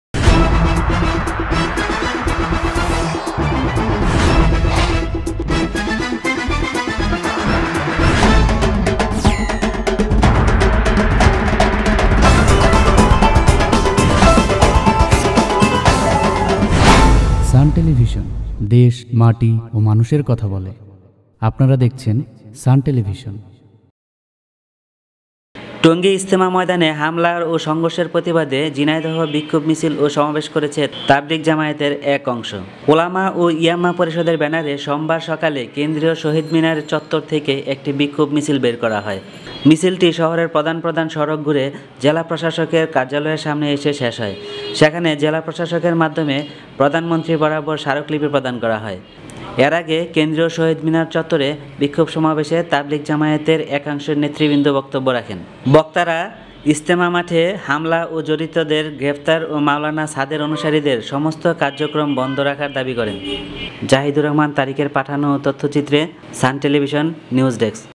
TV Channel-Sun Television
টঙ্গীর ইজতেমা ময়দানে হামলা ও সংঘর্ষের প্রতিবাদে ঝিনাইদহে বিক্ষোভ মিছিল ও সমাবেশ করেছে তাবলীগ জামায়াতের একাংশ